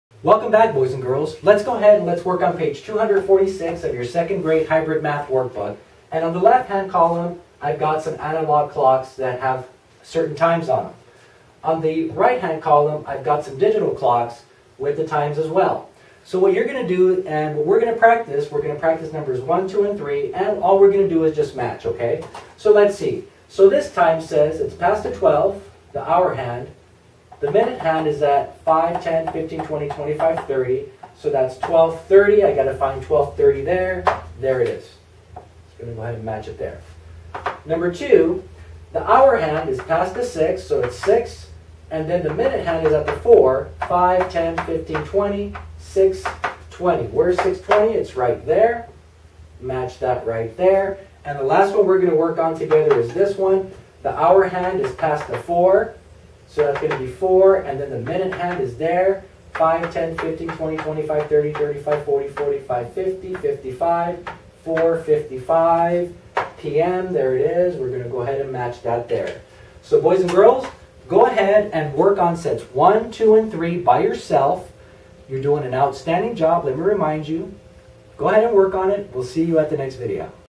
2nd Grade Math: Matching Analog Clock Times to Digital Clock Times
Second Grade Common Core Math can be frustrating for both students and parents.  This is why Noel Morales, M.Ed. will help you understand what these new expectations are and how you can help your second grade child at home. In this video he will work with your child on matching analog clock times to digital clock times. Questions? Like what you see?